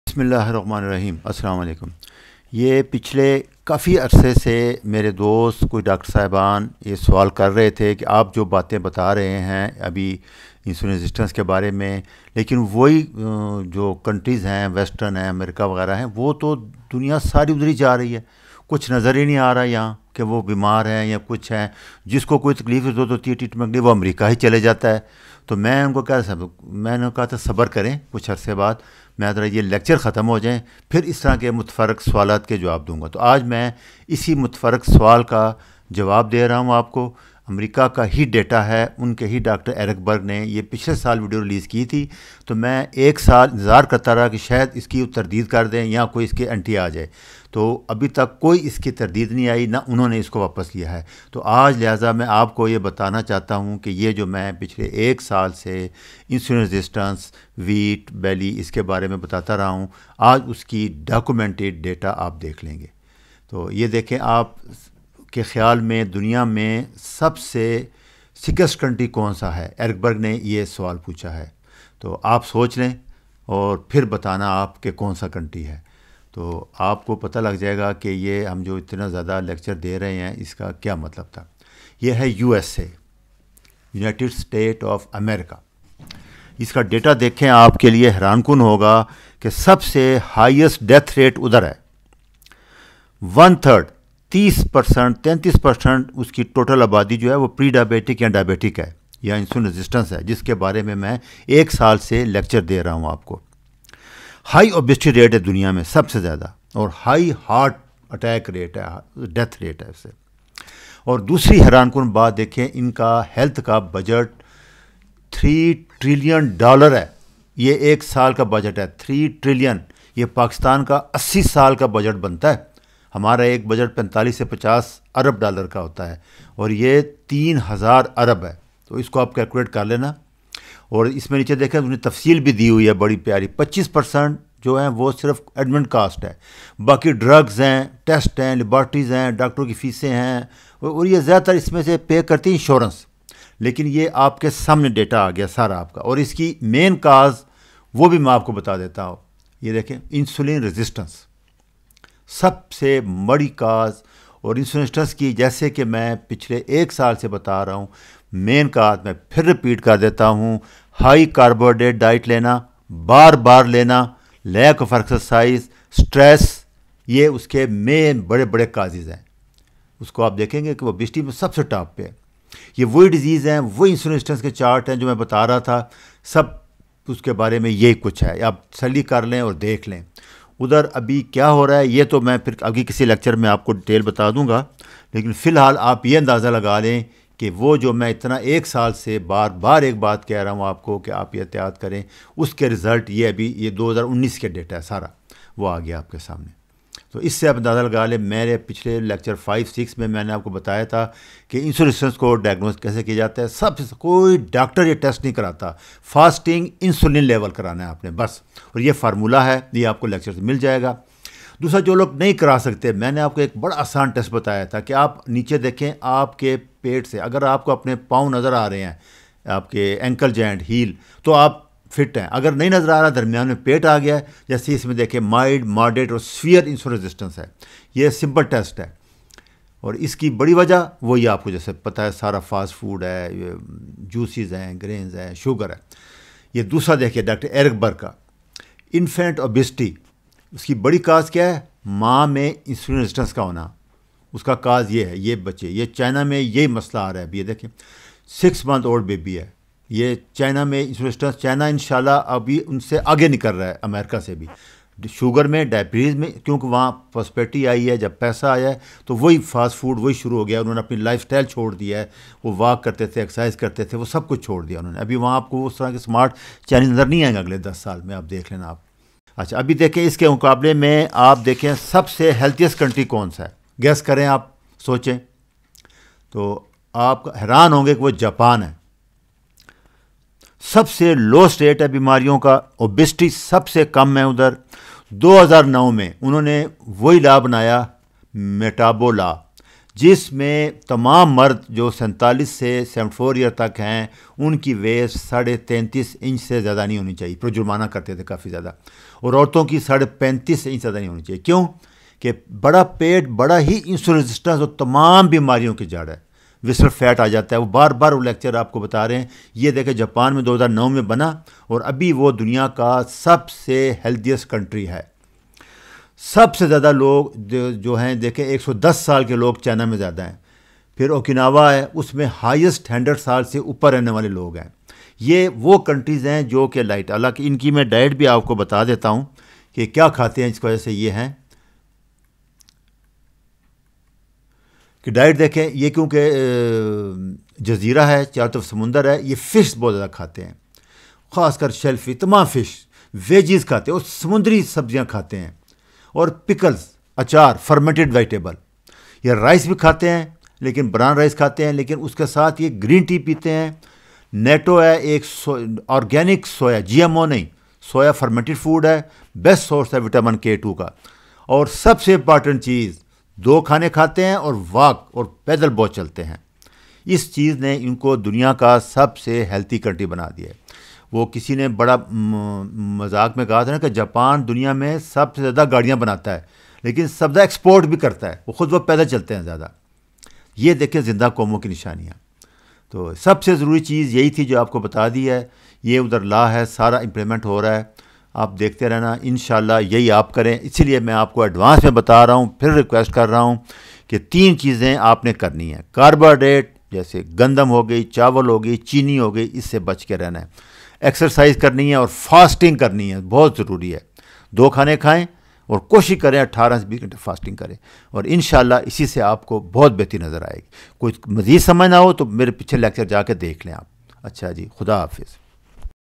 World's Sickest and Healthiest Country? - Lecture 32
Which is the sickest country in the World? Which is the healthiest country in the world and Why?

Find the answer to this interesting question in this video